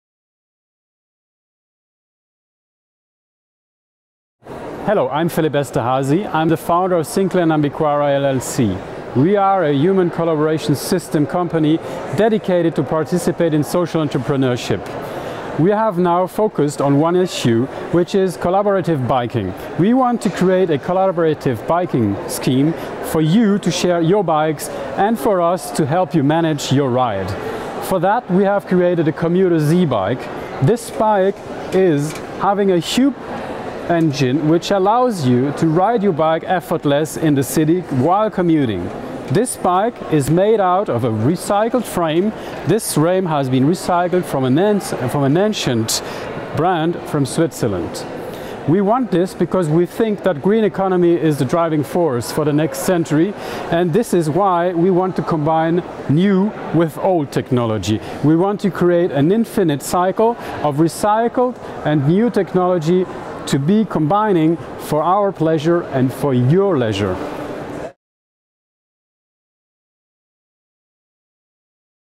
Swap&Drive @SIGEF2015 by Horyou
SIGEF2015 by Horyou gathered participants from all corners of the world for three days in Geneva, Switzerland in an effort to shape better times to come.

About 100 non-profit organizations, 50 socially innovative project holders, 60 international speakers and panelists over 8 plenary sessions and 15 theme panels took place from Oct 23-25 at Bâtiment des Forces Motrices. That's what SIGEF2015 was about; compassionate people who, with their dreams, inspirations and actions are seeking ways to come to grips with the complexities of our world, and not give up before the difficulties and obstacles that they encounter. See their joy to live and give.

In this video, Philip Esterhazy presents the project Swap&Drive.
Swap&Drive is about collaborative biking and retrofit to electric, recycled swiss military bike frames.